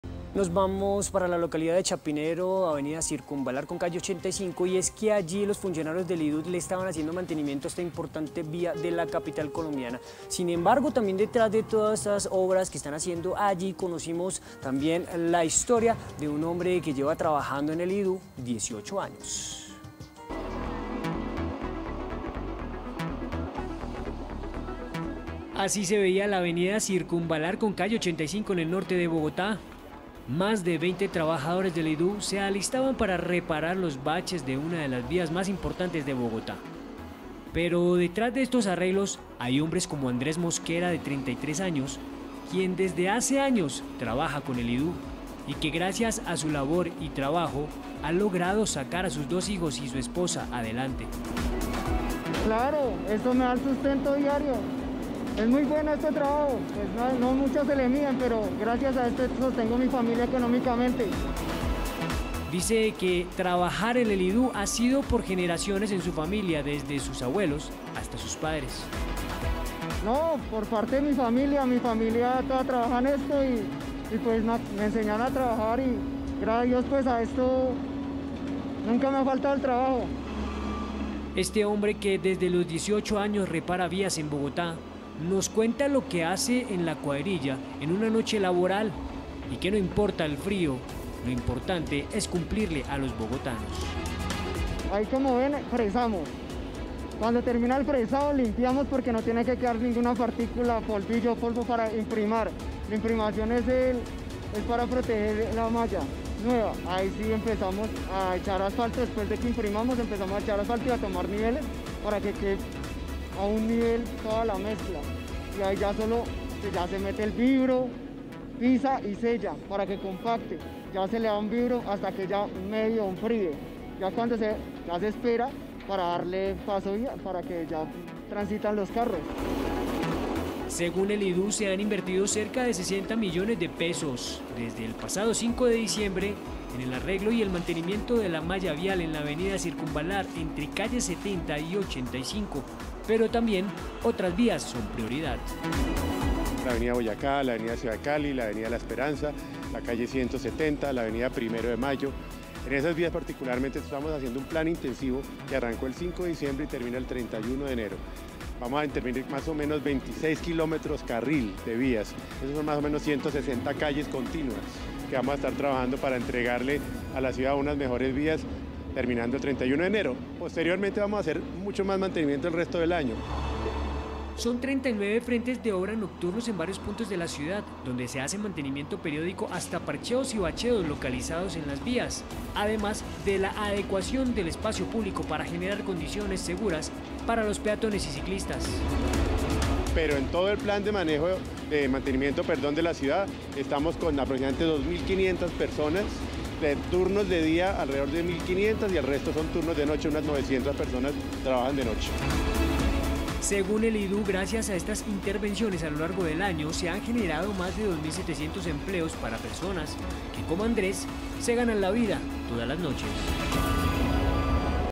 De noche, 900 personas arreglan la malla vial | El Tiempo
Trabajos nocturnos del IDU. Mientras la mayoría de la ciudad duerme, por lo menos 900 personas en 39 frentes de obra trabajan en el arreglo de la malla vial de la ciudad. El equipo noctámbulo acompañó la intervención de la avenida circunvalar con calle 85.